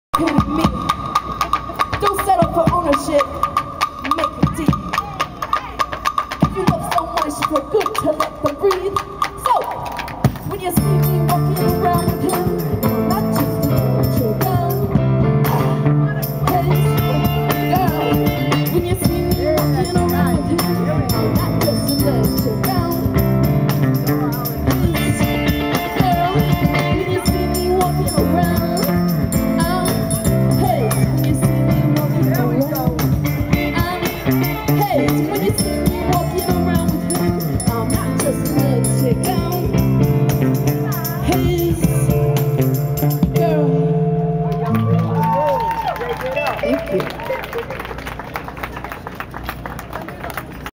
Dancing Idiots' Waltz in F(ucktard) Major
Relegating the Dirty Old Man to the background of this quickie are the off-key singer's mom, the Wacky Business Associate, and a couple of others who danced quite idiotically, and from whom I needed to quickly back away to avoid getting knocked over. Sadly, this clip is some of the best material I have as far as that band goes. They sucked that much.